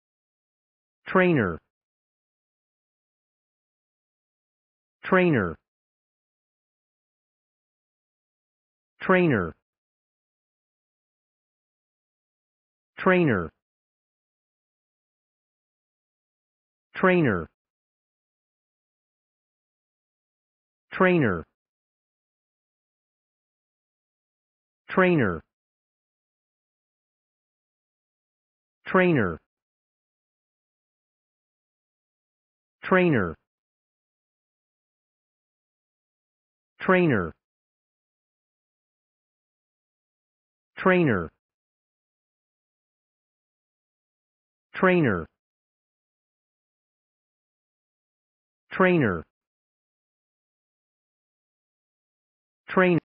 Pronunciation of Trainer | Definition of Trainer
Trainer pronunciation | How to pronounce Trainer in English?
/`treɪnər/
Meaning of Trainer | What is Trainer?
(1) (noun) simulator consisting of a machine on the ground that simulates the conditions of flying a plane; (2) (noun) one who trains other persons or animals;
What is Trainer | Trainer meaning and definition?